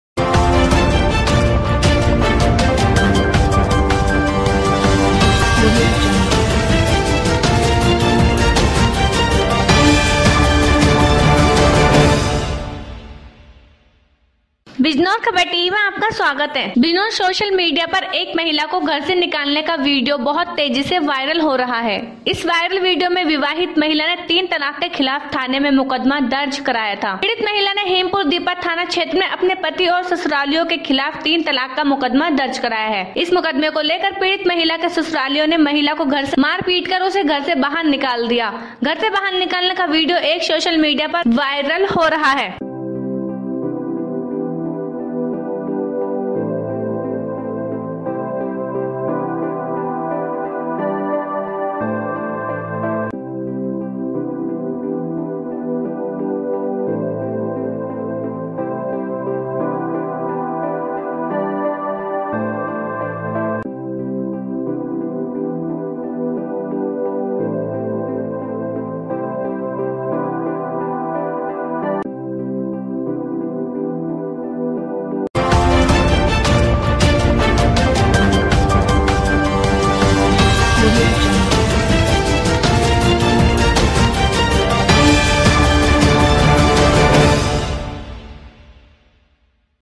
BIJNOR KHABAR || बिजनौर वायरल वीडियो में महिला की लाइव पिटाई
बिजनौर।सोशल मीडिया पर एक महिला को घर से निकालने का वीडियो बहुत तेजी से वायरल हो रहा है।इस वायरल वीडियो में विवाहित महिला ने तीन तलाक के खिलाफ थाने में मुकदमा दर्ज कराया था। पीड़ित महिला ने हीमपुर दीपा थाना क्षेत्र में अपने पति और ससुरालियों के खिलाफ तीन तलाक का मुकदमा दर्ज कराया है ।इस मुकदमे को लेकर पीड़ित महिला के ससुरालियों ने महिला को घर से मारपीट कर उसे घर से बाहर निकाल दिया।घर से बाहर निकालने का वीडियो एक सोशल मीडिया पर वायरल हो रहा है।